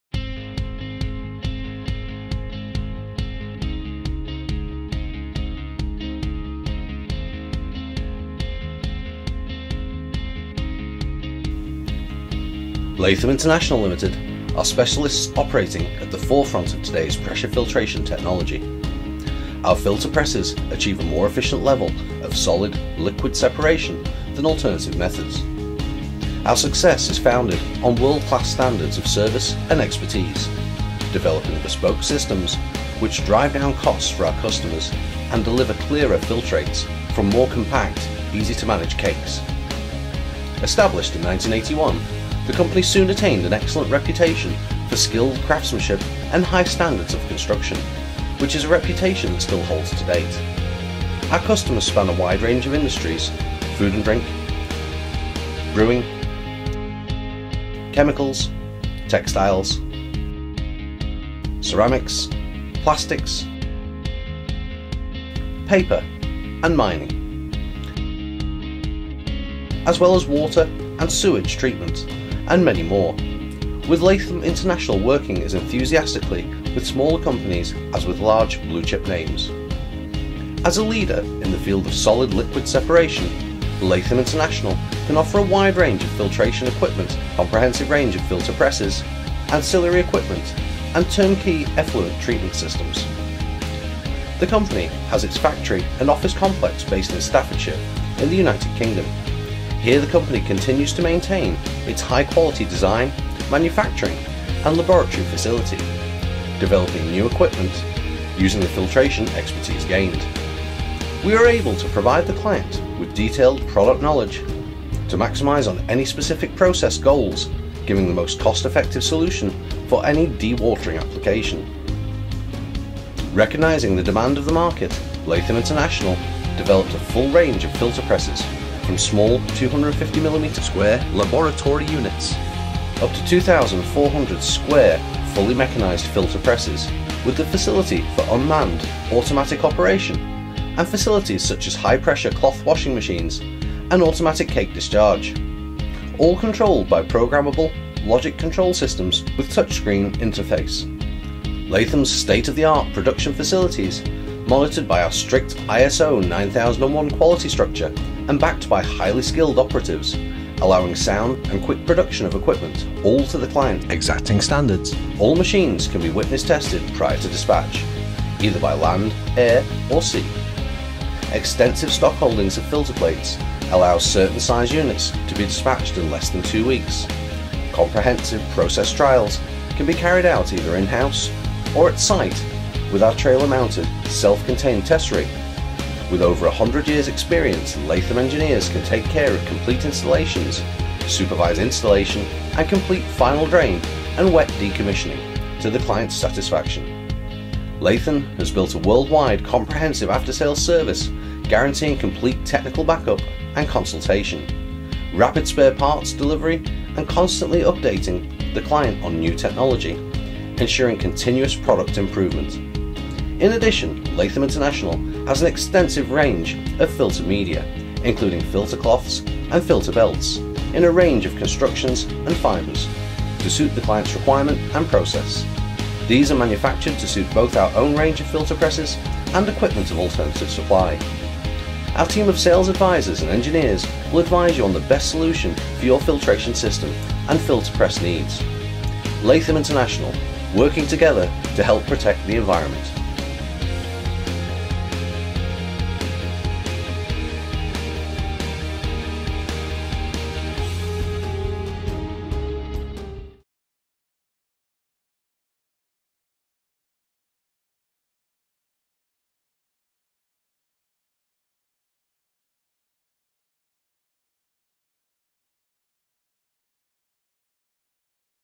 Latham International Filter Press Manufacturer
General Introductory video giving a general overview of the services and products offered by Latham International.  Including Filter Presses, Filter Plates, Filter Cloths, Filter Press Feed Pumps.